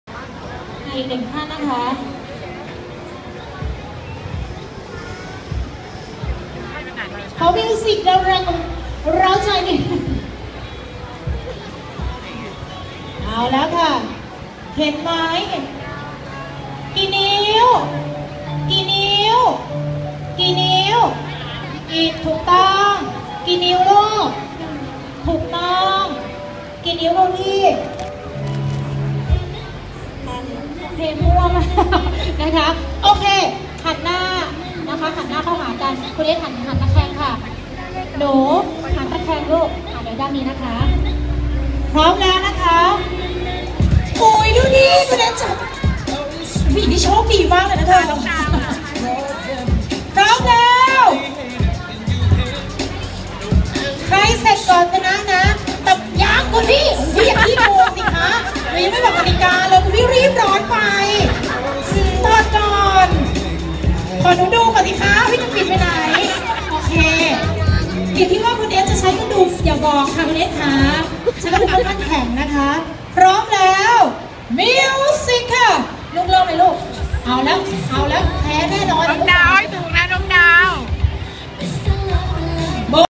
เอส กันตพงศ์ เล่นเกมส์ กับแฟนคลับ @ โรบินสัน กาญจนบุรี
งาน Jeans Most Wanted
โรบินสัน กาญจนบุรี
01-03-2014